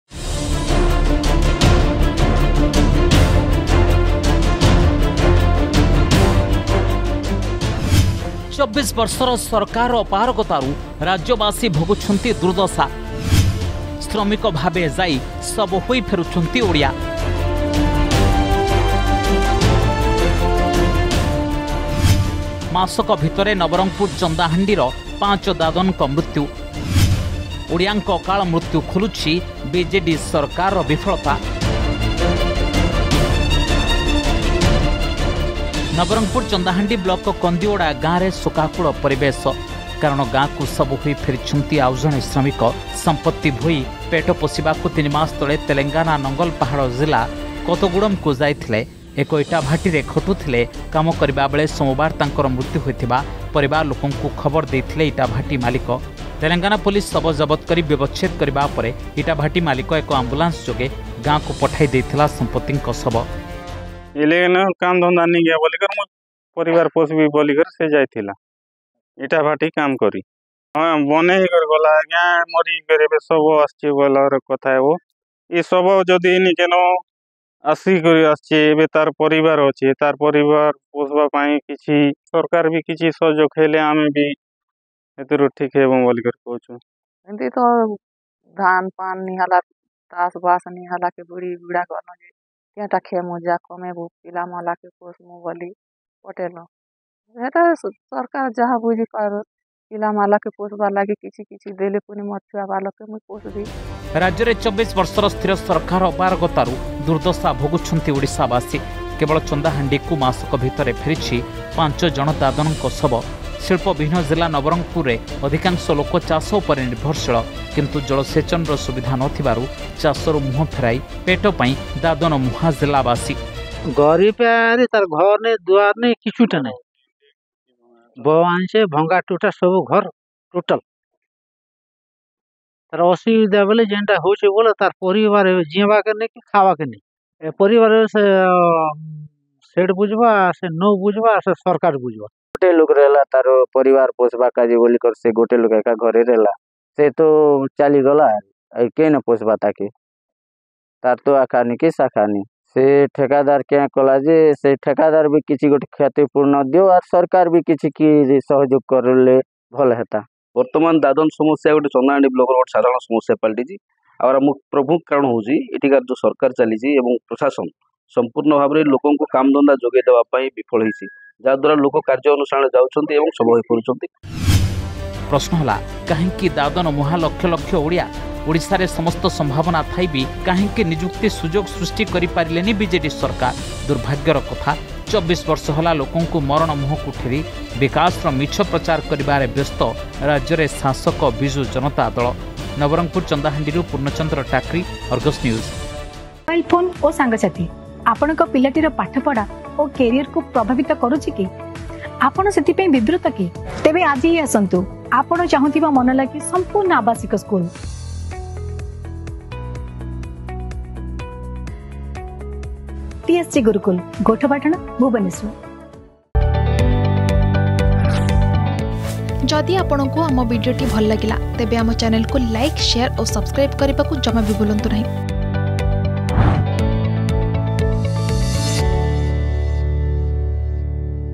ଥମୁନି ଦାଦନ ଯନ୍ତ୍ରଣା || Odia Migrant Labourers Death Case
୨୪ ବର୍ଷର ସରକାରର ଅପାରଗତାରୁ ରାଜ୍ୟବାସୀ ଭୋଗୁଛନ୍ତି ଦୁର୍ଦ୍ଦଶା । ଶ୍ରମିକ ଭାବେ ଯାଇ ଶବ ହୋଇ ଫେରୁଛନ୍ତି ଓଡ଼ିଆ । ମାସକ ଭିତରେ ନବରଙ୍ଗପୁର ଚନ୍ଦାହାଣ୍ଡିର ୫ ଦାଦନଙ୍କ ମୃତ୍ୟୁ । ଓଡ଼ିଆଙ୍କ ଅକାଳ ମୃତ୍ୟୁ ଖୋଲୁଛି ବିଜେଡି ସରକାରର ବିଫଳତା ।